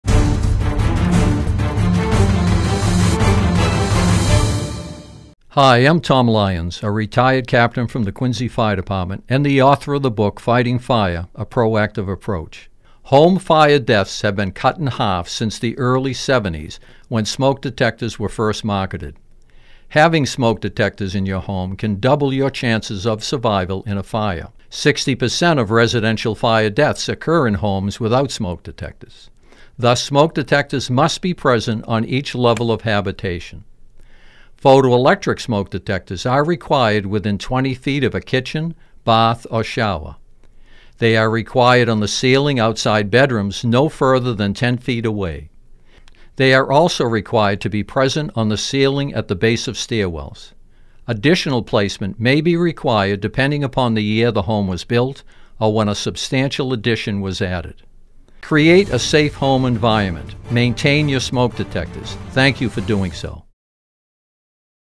Fighting Fire: Smoke Detectors
Retired Quincy Fire Department Captain Tom Lyons shares life-saving tips. The fourth in a series of PSAs.